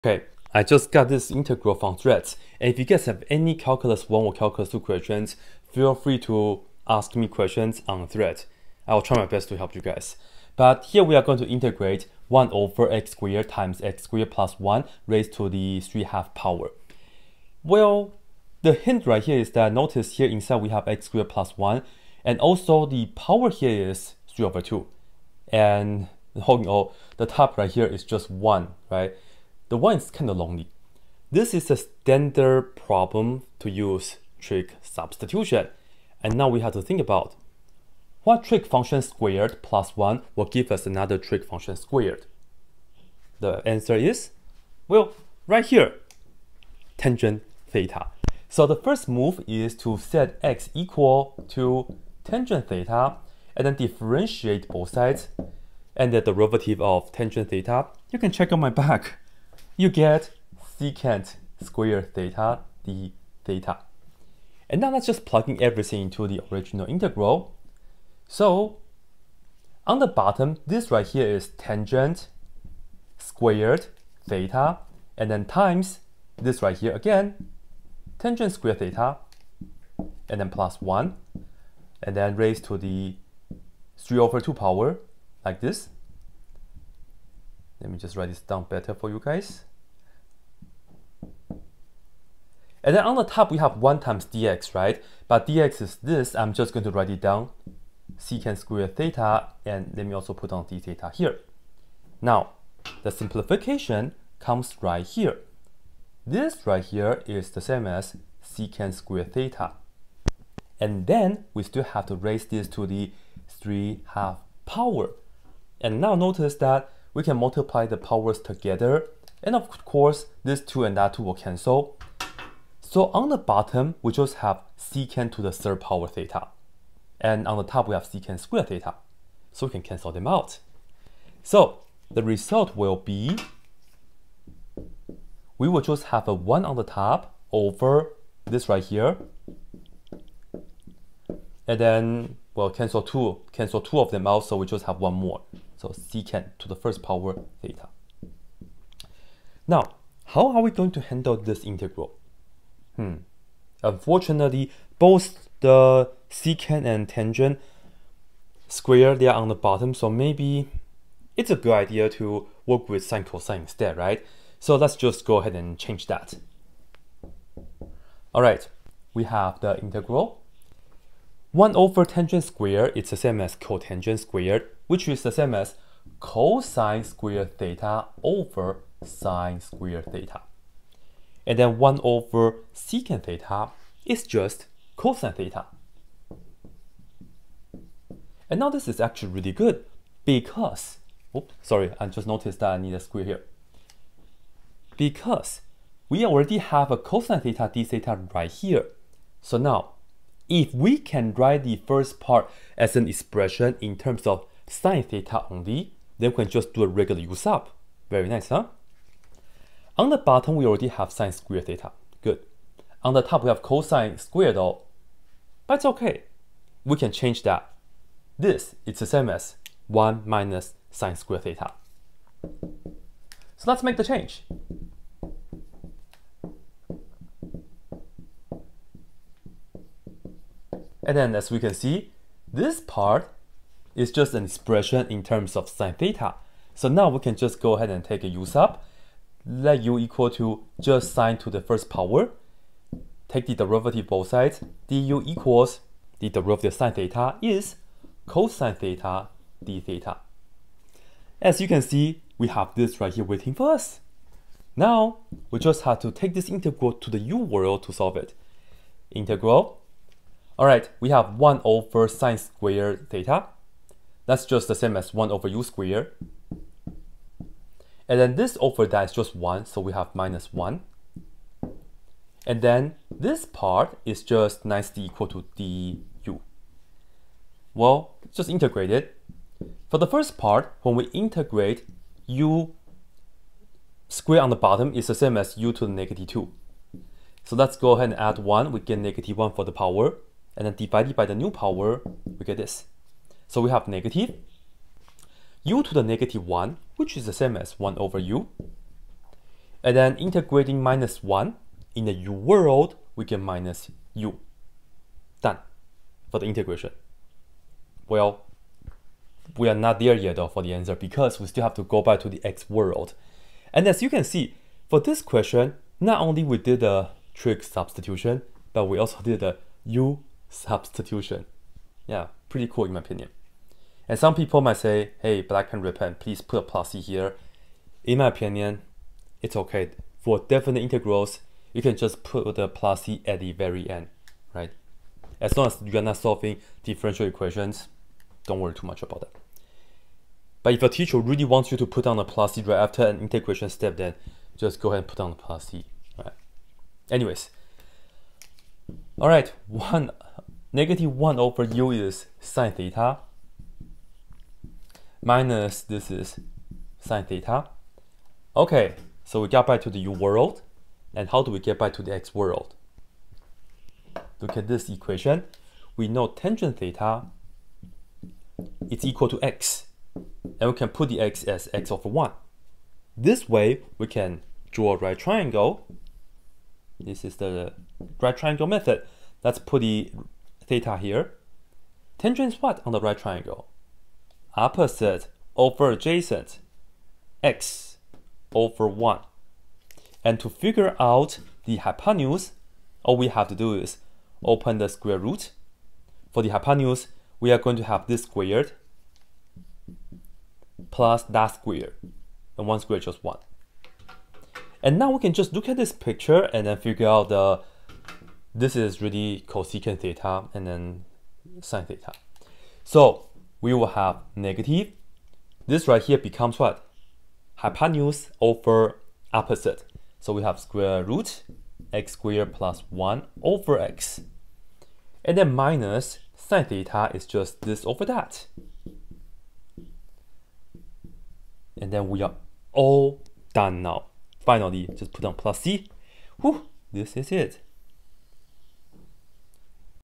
Helping you with the integral of 1/(x^2*(x^2+1)^(3/2)) using trigonometric substitution, Calculus 2
Learn how to use trigonometric substitution for the integral of 1/(x^2*(x^2+1)^(3/2)). This question is from Threads